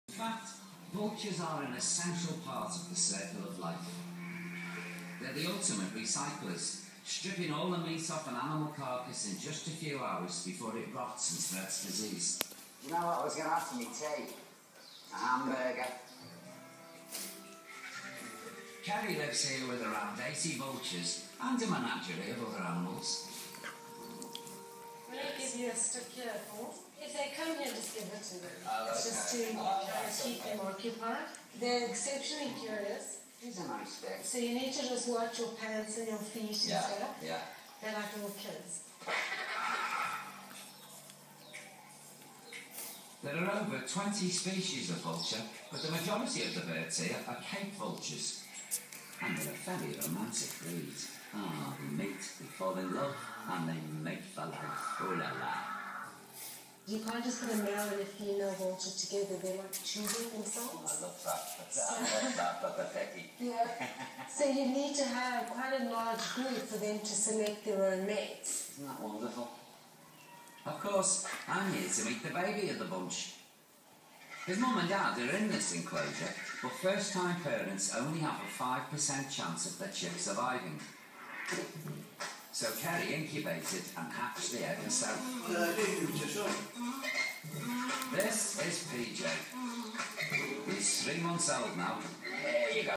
Ozzi And Tootsie Kitten And Dog Playing
Each Cat & Kitten at Animal Guardians Malta Sanctuary has a Special Story .so have the cats & kittens who were adopted into Loving, Forever homes. This is Ozzy...adopted at the age of 10 weeks , about two months ago. All the family Love Ozzy...including sweet Tootsie ...Thank You to all of you beautiful people who adopt